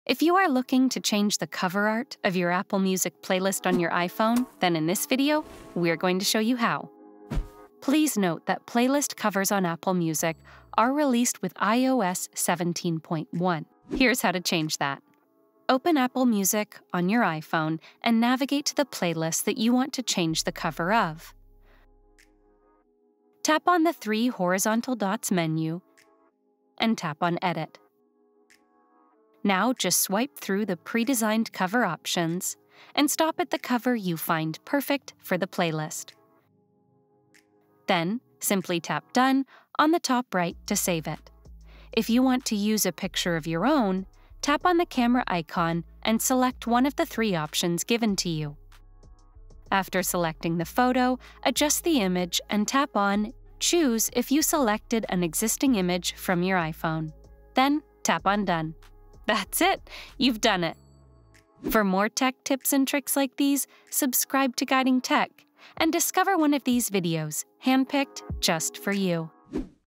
How To Change Playlist Cover on Apple Music on iPhone | Customise Your Apple Music Playlist Cover!
If you are looking to change the cover art of your Apple Music playlist on your iPhone, then in this video we are going to show you how!

You can not only add any image to the playlist cover on Apple Music but with the new iOS17.1 update, you can also add new pre-designed covers to your playlist on iPhone. Please note that playlist covers on Apple Music are released with iOS 17.1. Here’s how to change that.